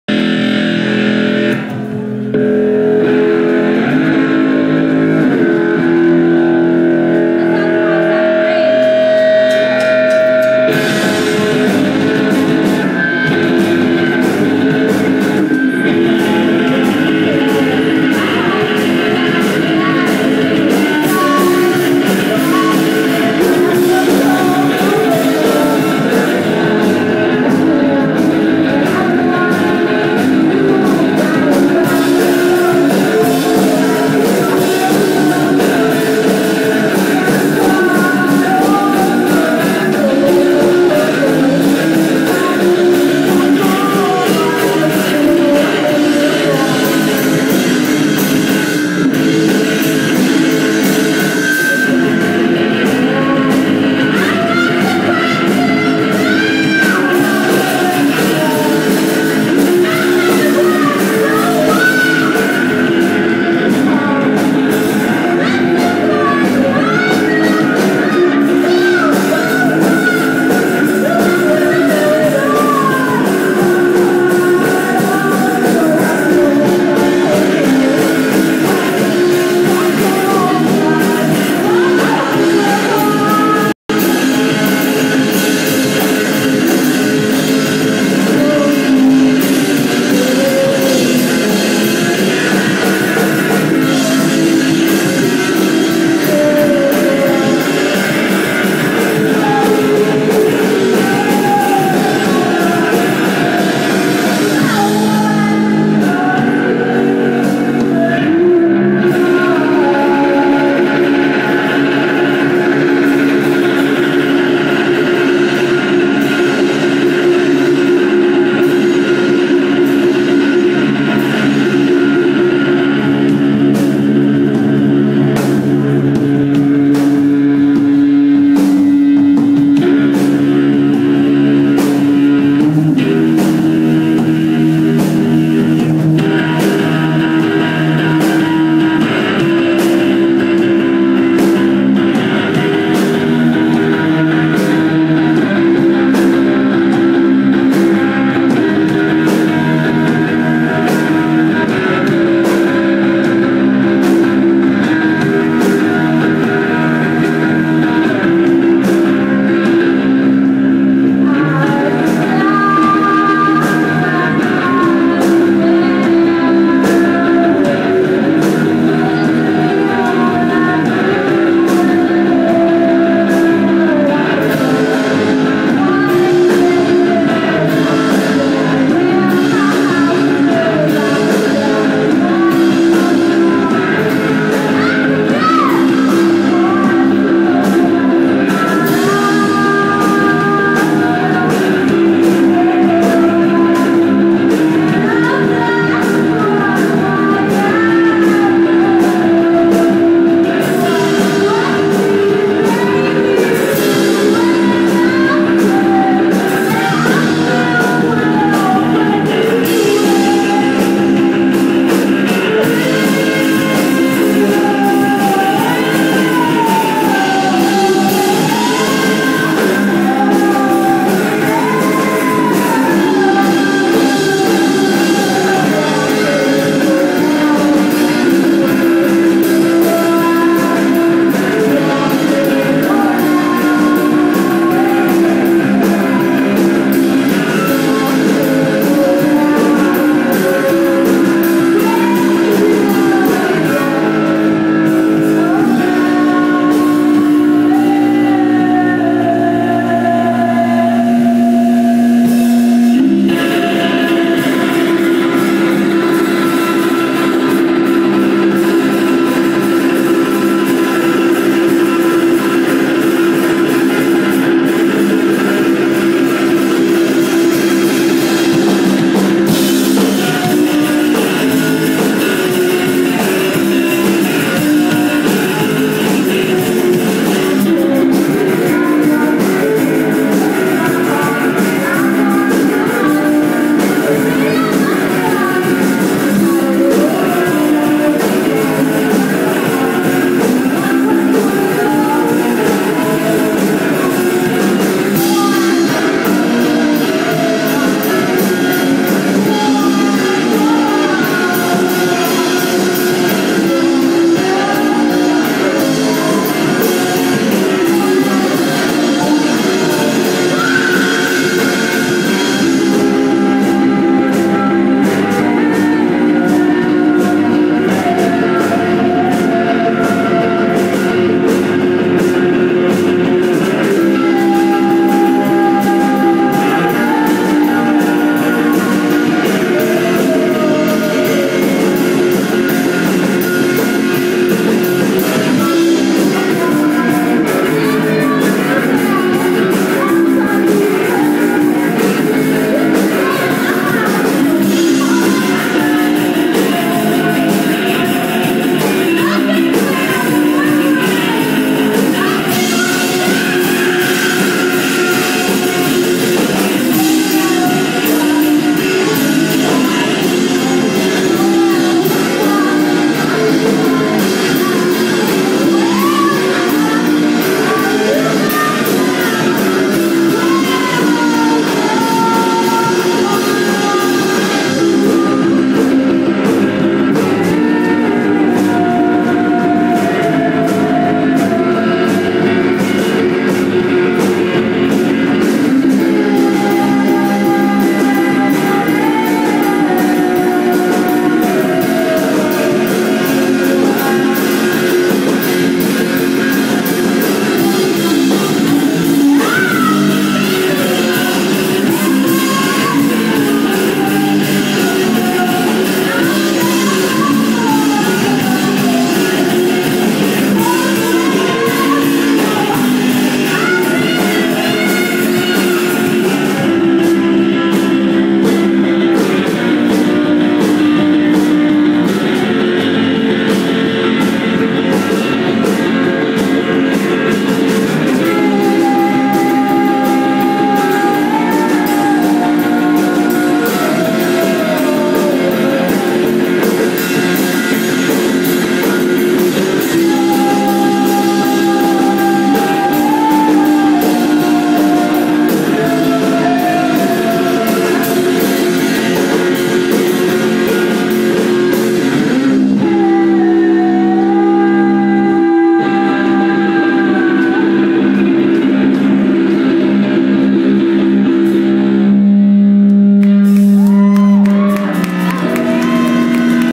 SLUTEVER, Live @ The Level Room Philadelphia 123111
SLUTEVER plays a 3 song stretch in this video for the NYE show at The Level Room in Philly  ** almost no lighting to work with on this video...